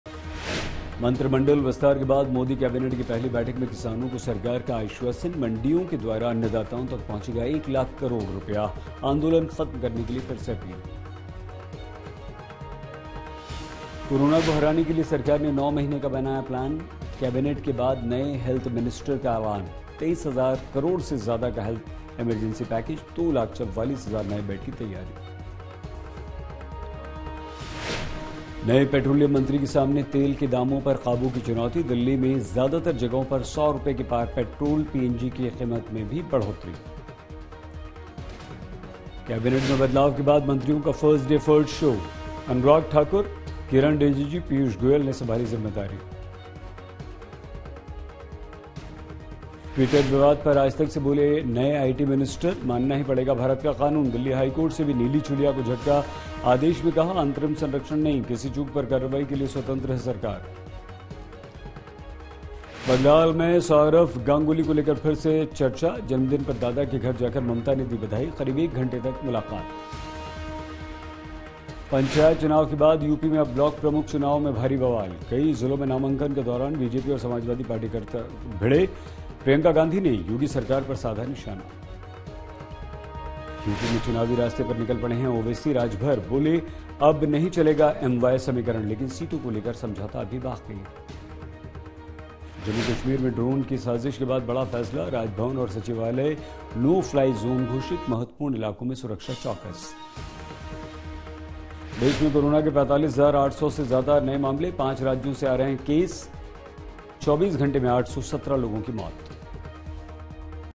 'किसानों तक पहुंचेगा 1 लाख करोड़' सरकार ने किसानों को दिलाया भरोसा | देखें दिनभर की 10 बड़ी खबरें
Modi cabinet Expansion 2021: केंद्रीय Agriculture Minister Narendra Singh Tomar ने प्रेस ब्रीफिंग को संबोधित करते हुए कहा कि एपीएमसी मंडियों को और मजबूत किया जा रहा है. कृषि मंडियों को और संसाधन दिया जाएगा. मंडियां खत्म नहीं होने वाली हैं. तोमर ने कहा, ''हमारे देश में एक बड़ा क्षेत्र है, जहां नारियल की खेती होती है. नारियल का प्रोडक्शन बढ़े, इसके लिए नारियल बोर्ड 1981 में बना था. इस बोर्ड में सरकार संशोधन करने जा रही है. बोर्ड का अध्यक्ष किसान पृष्ठभूमि से होगा और वह जमीन की हालात को सही से समझ सकेगा. इसके अलावा, एग्जीक्यूटिव पावर के लिए एक सीईओ बनाया जाएगा. बोर्ड में दो तरह के सदस्य होंगे.'' केंद्रीय कृषि मंत्री ने बताया कि मंडियां इंफ्रास्ट्रक्चर फंड का इस्तेमाल कर सकेंगी...